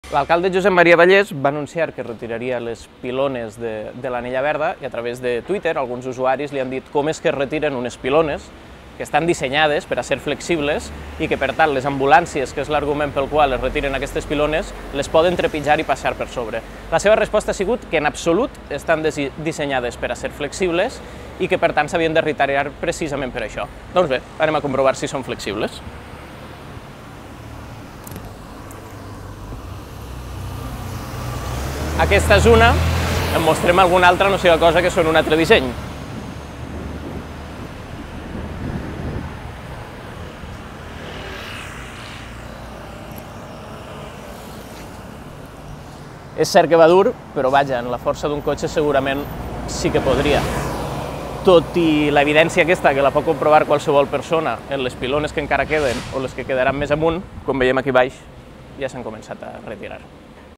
Les pilones de l'anella verda són flexibles
El govern havia anunciat la seva retirada perquè impedien l'accés de vehicles d'emergència.